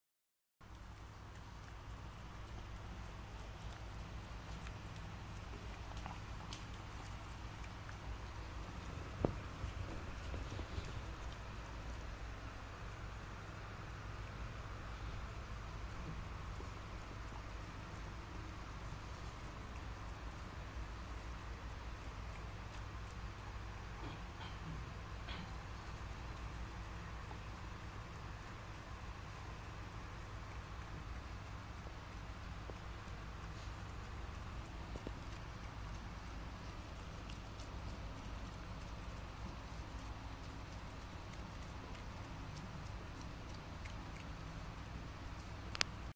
คอร์กี้ Eat Tofu. Welsh Corgi, 'Giraffe'
'Giraffe' the Corgi dog.
4 month old